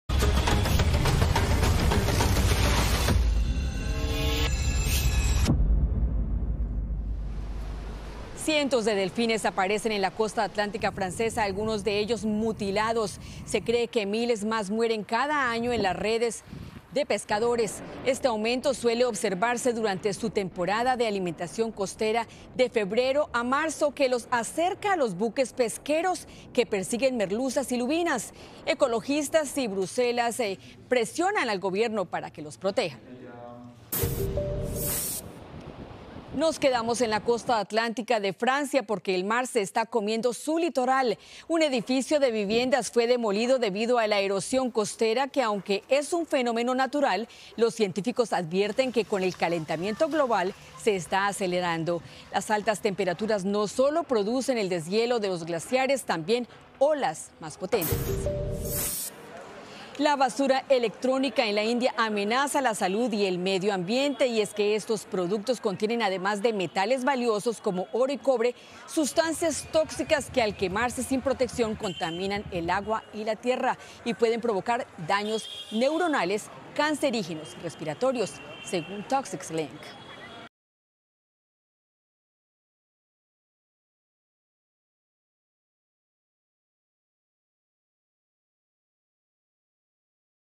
Planeta Tierra: Descubren los cadáveres de cientos de delfines en Francia | Noticias Telemundo
Video oficial de Noticias Telemundo. El incremento de los decesos se produce de febrero a marzo, la temporada de alimentación costera de estos animales. Se cree que cada año miles de ellos pierden la vida por causa de las redes de los buques pesqueros.